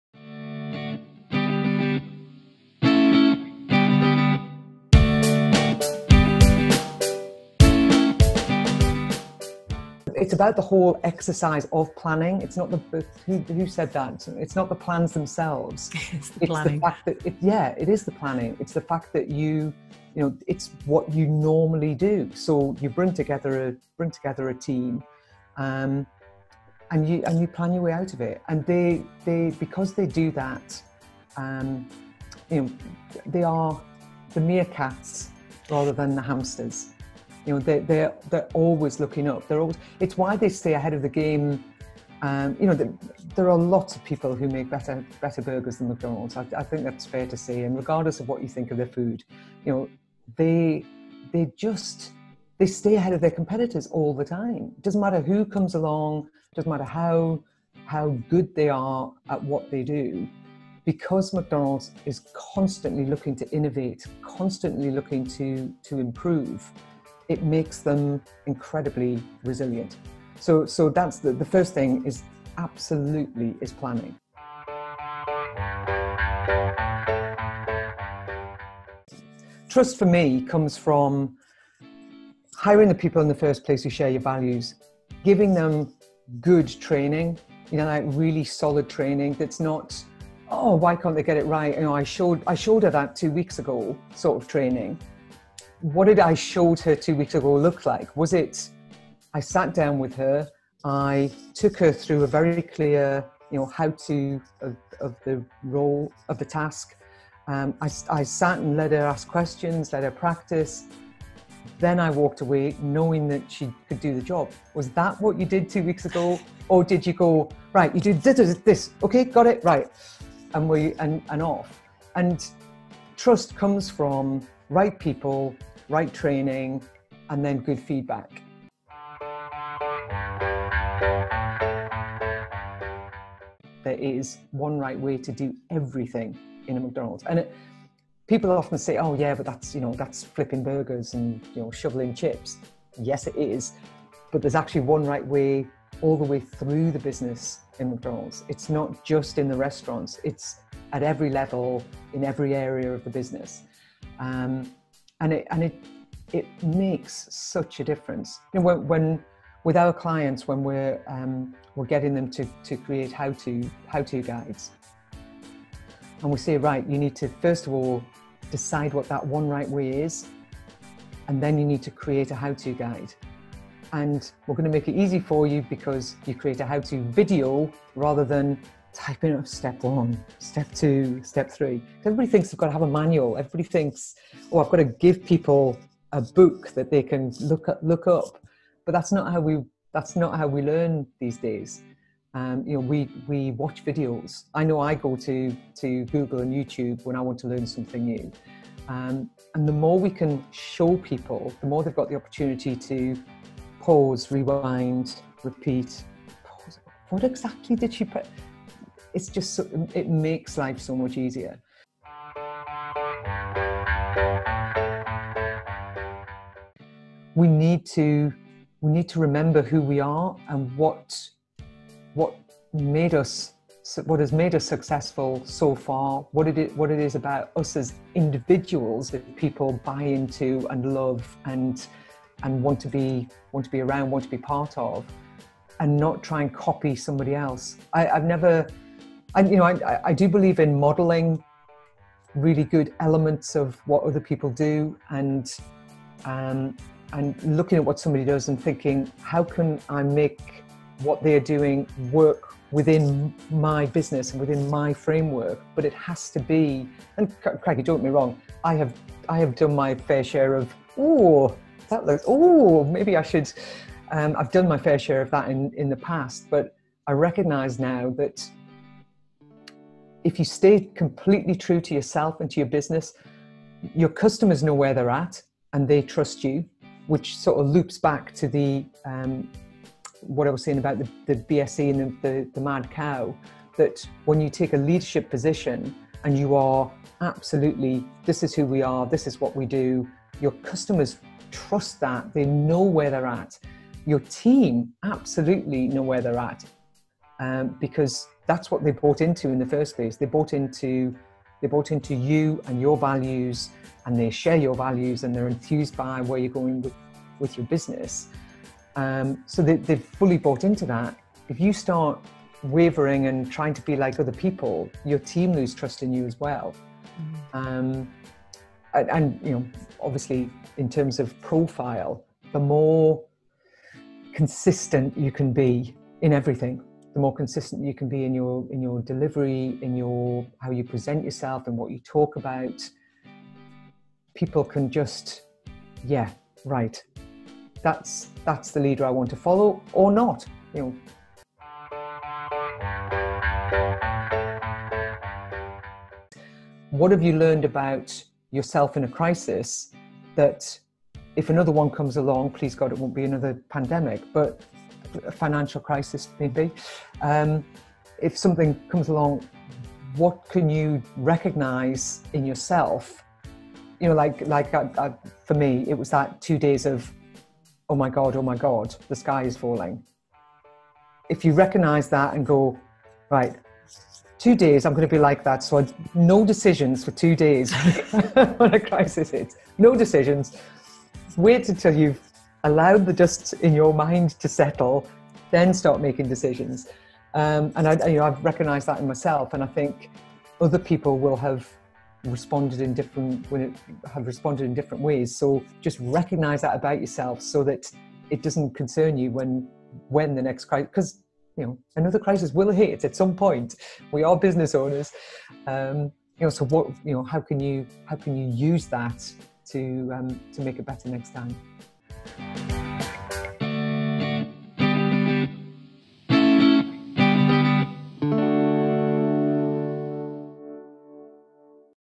PI-Q Webinar Marianne Page Best Bits
Highlights from my conversation on building a resilient business with Marianne Page, author of Simple, Logical, Repeatable and Mission: To Manage.